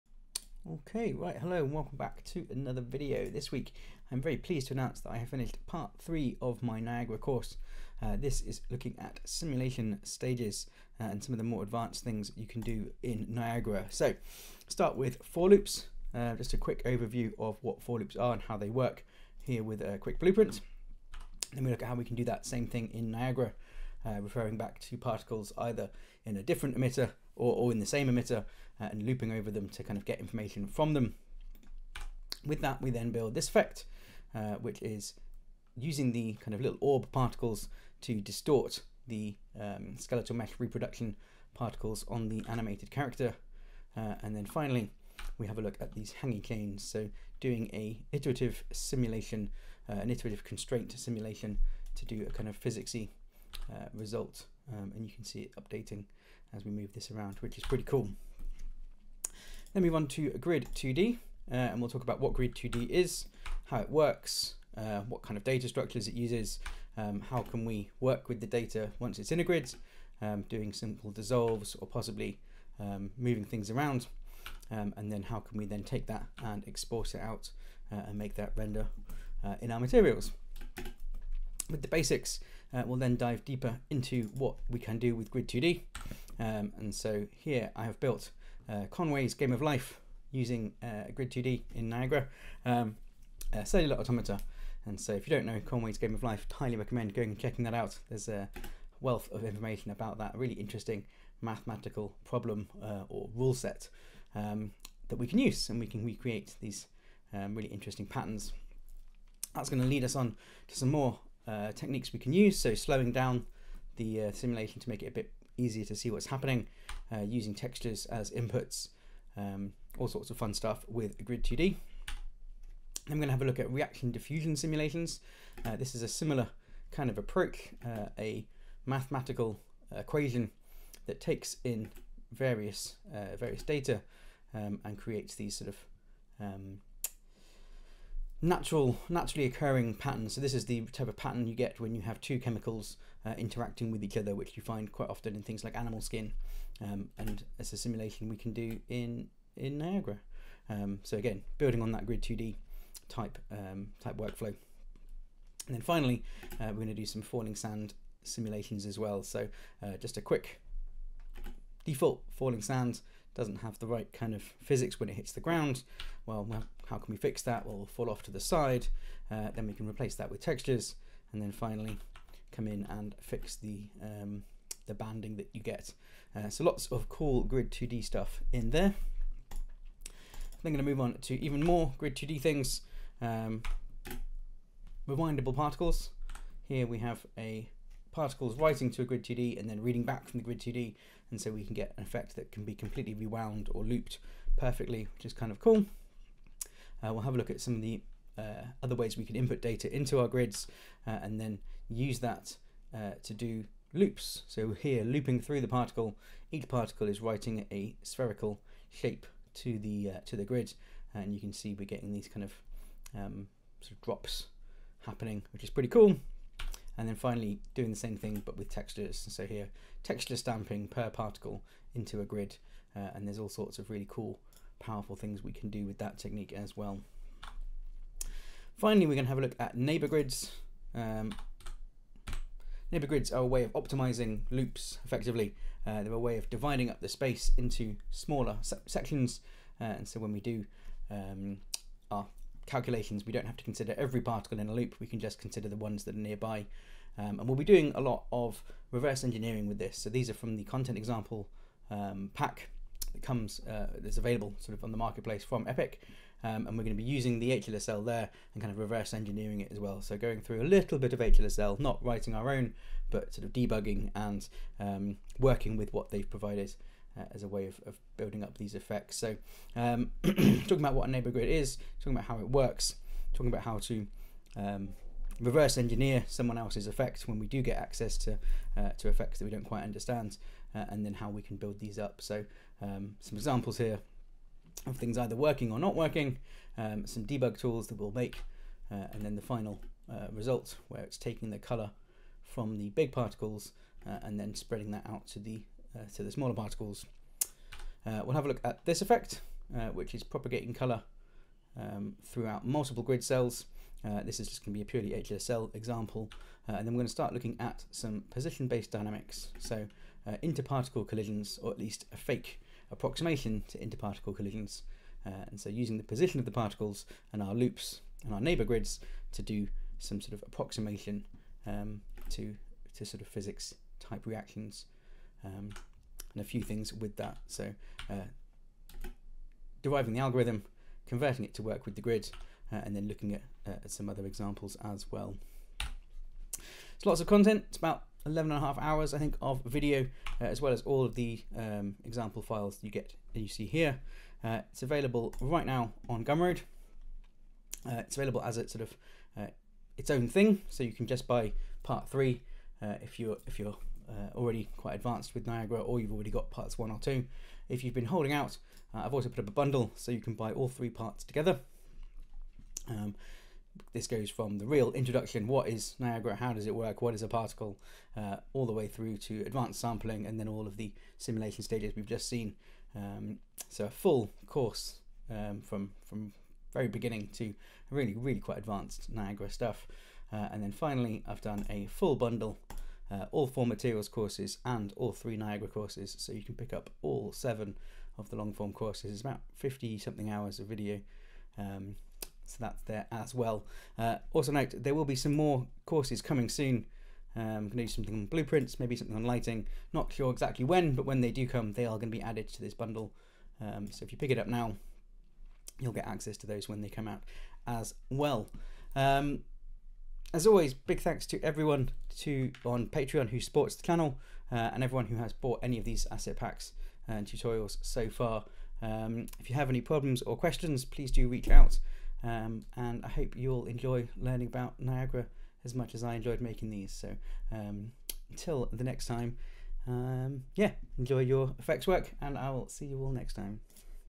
Niagara Simulation Stages Course - Now Available!
This week I'm very happy to announce that part 3 of my Niagara course is now available - covering For Loops, Simulation Stages, Grid2D and Neighbour Grids, this is some of the most advanced Niagara techniques you can use!

Or you can find more of my work on my